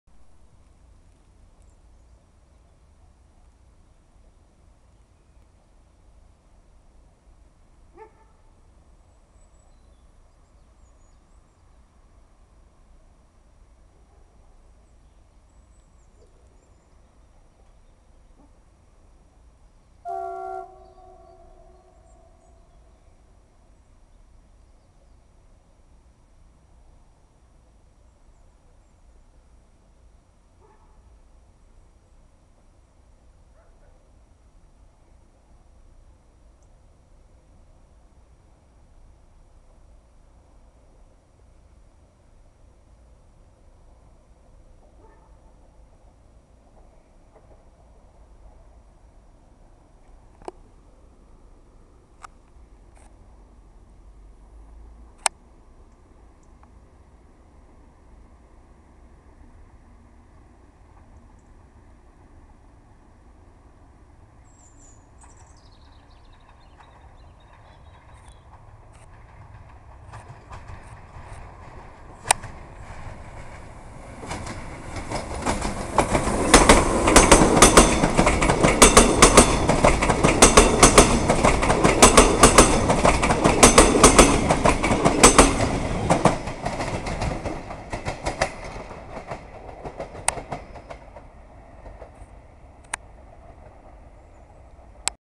KM Mazovia - Gostynin - E583 - Zacne dźwięki
Wieczorny przelot pociągu przyśpieszonego relacji Wwa Wschodnia-Płock. Podstawili tyle piętrusów,a w pociągu frekwencja chyba tylko 5%...niedługo kurs do Płocka zapewme zlikwidują :( a to zawsze jakaś odmiana od tych wszechobecnych VT627 , VT628 i SA135...